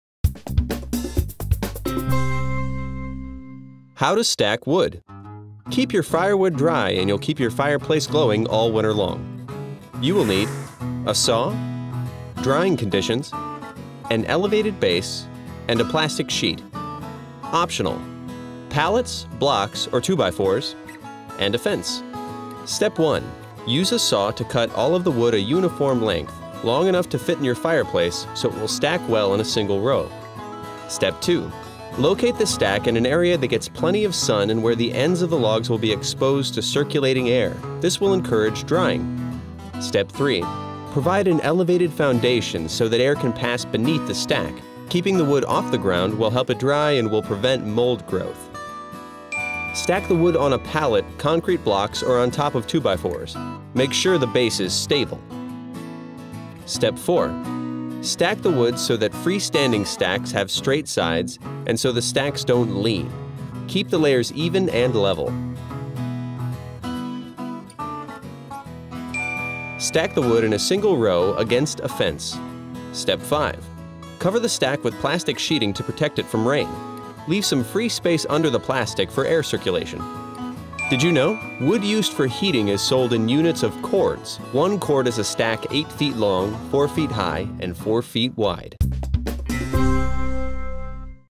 How to Stack Wood
Keep your firewood dry, and you'll keep your fireplace glowing all winter long.

Step 1: Cut the wood the same length
Use a saw to cut all of the wood a uniform length – long enough to fit in your fireplace – so it will stack well in a single row.

Step 2: Encourage drying
Locate the stack in an area that gets plenty of sun and where the ends of the logs will be exposed to circulating air. This will encourage drying.

Step 3: Provide an elevated base
Provide an elevated foundation so that air can pass beneath the stack. Keeping the wood off the ground will help it dry and will prevent mold growth.

Tip
Stack the wood on a pallet, concrete blocks, or on top of 2-by-4s. Make sure the base is stable.

Step 4: Keep stack layers level
Stack the wood so that freestanding stacks have straight sides, and so the stacks don't lean. Keep the layers even and level.

Tip
Stack the wood in a single row against a fence.

Step 5: Cover the stack
Cover the stack with plastic sheeting to protect it from rain. Leave some free space under the plastic for air circulation.

Wood used for heating is sold in units of cords. One cord is a stack 8 feet long, 4 feet high, and 4 feet wide.